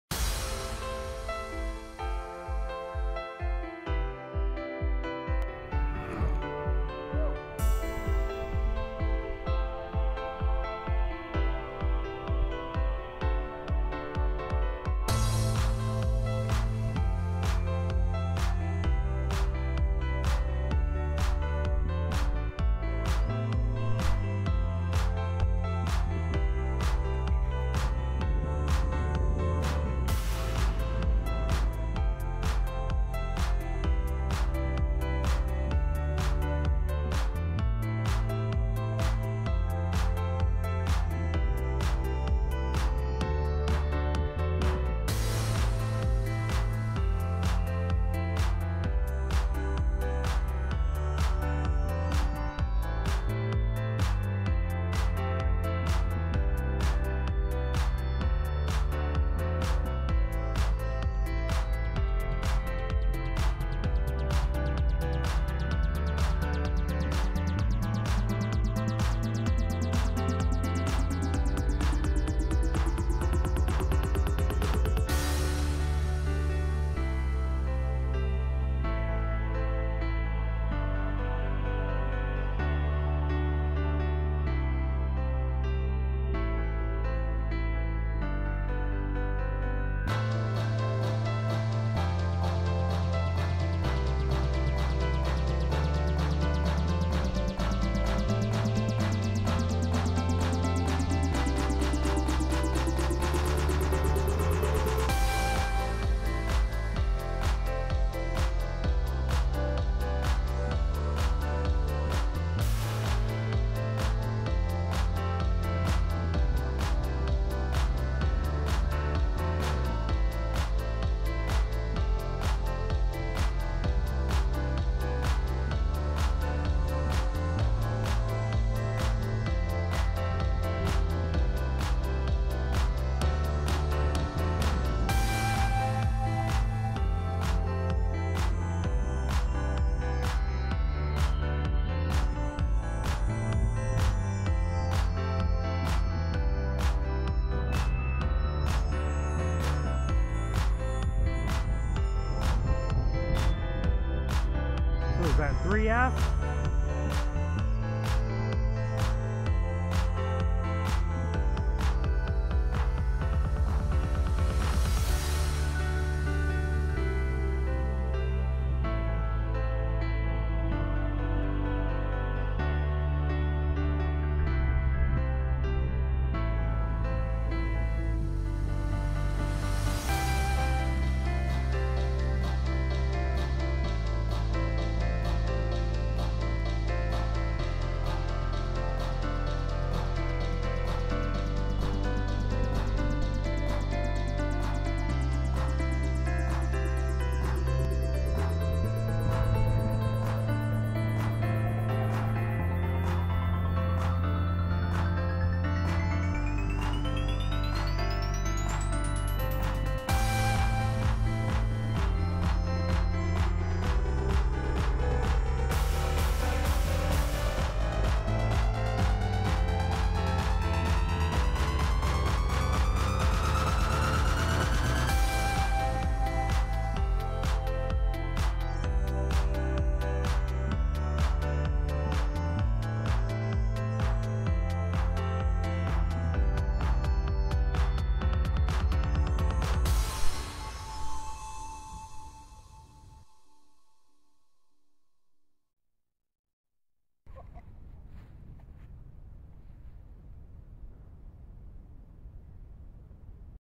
Good Lookin Mike got a New Valiant
SlowJo caught up with Mike at Tulip City Air Force, to film The new E-flite 1.3m Valiant he picked up at Cobblestone Crafts and Hobby in Holland Mi., Beautiful plane with a wide flite envelope, video shot with RunCam2 4K, edited on HitFilm, With help from Northern Viking Everyday